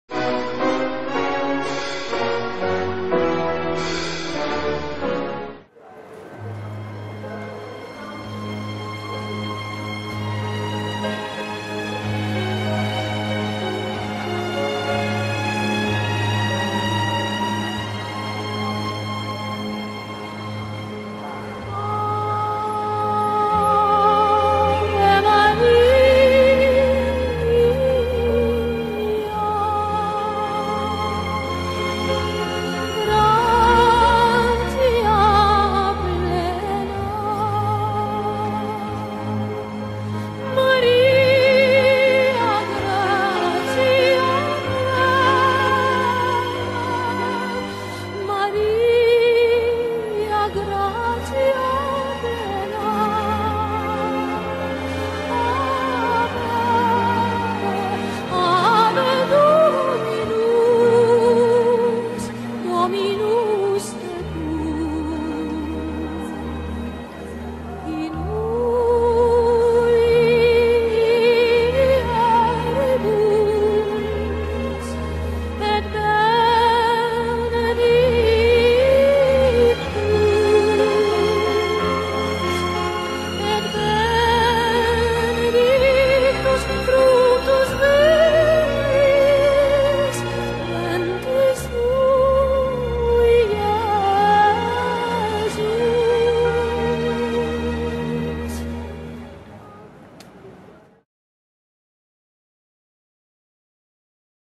Tribuna de Macau TV - Procissão 13 Maio (2013)
A comunidade católica reuniu-se  novamente para percorrer as ruas da cidade em homenagem a Nossa Senhora de Fátima. Foram várias centenas as pessoas que professaram a sua fé no 13 de Maio, durante uma procissão guiada por três pastorinhos